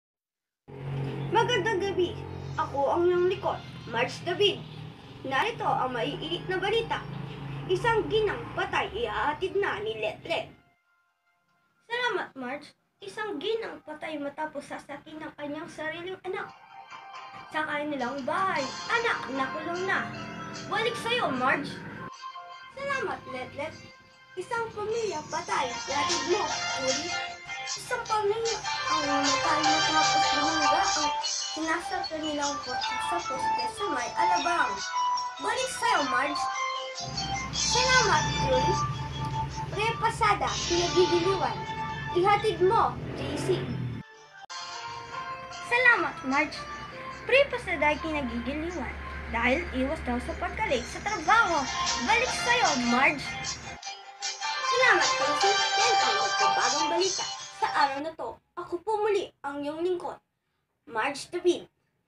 Broadcasting
A project by my student Marjhorie David and as partial requirement on our subject Media and Information  Literacy.  This student made me proud of her because she was not able to join her group mates due to certain circumstances and yet she did her best to finish her broadcasting project on her own.  Kudos to you my dear student, you have shown me that no matter the struggle and difficulties you will face in studying you will still come up with your own way to do your task.  Thank you and God Bless you.